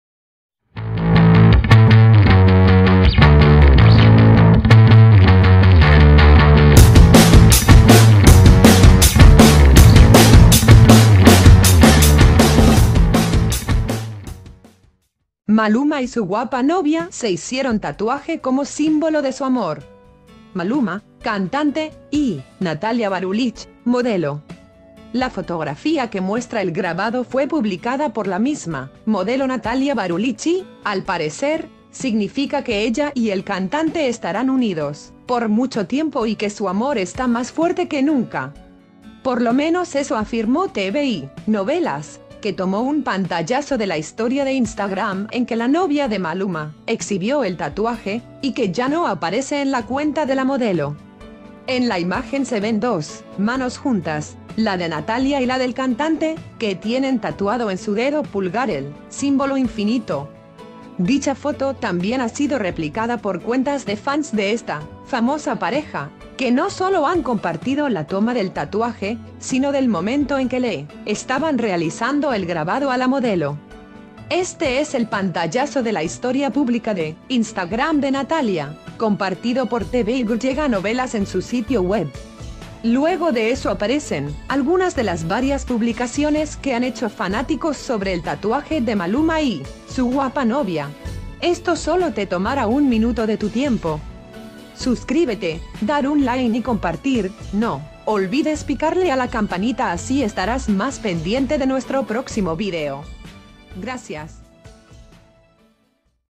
Maluma y su guapa novia se hicieron tatuaje como símbolo de su amor
La fotografía que muestra el grabado fue publicada por la misma modelo Natalia Barulích y, al parecer, significa que ella y el cantante “estarán unidos por mucho tiempo y que su amor está más fuerte que nunca”.

Por lo menos eso , que tomó un pantallazo de la historia de Instagram en que la novia de Maluma exhibió el tatuaje, y que ya no aparece en la cuenta de la modelo.

En la imagen se ven dos manos juntas, la de Natalia y la del cantante, que tienen tatuado en su dedo pulgar el símbolo “Infinito”.